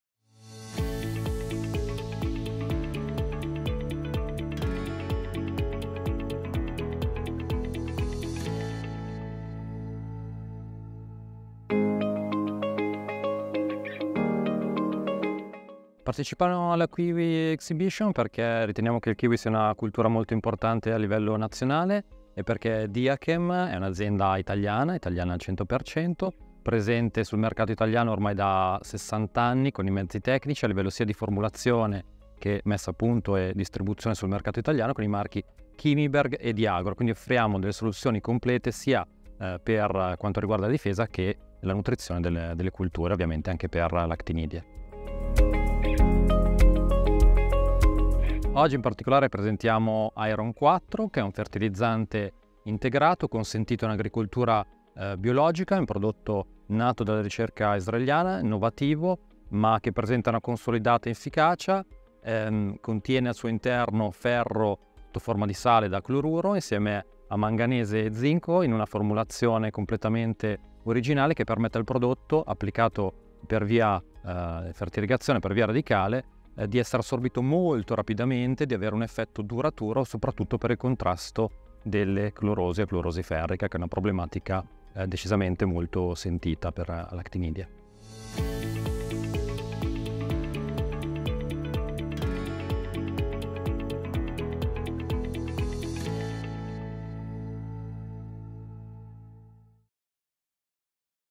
La Kiwi Exhibition secondo Diachem
Andrea Pedrazzini racconta perché partecipare alla Kiwi Exhibition e le soluzioni Diachem per il kiwi.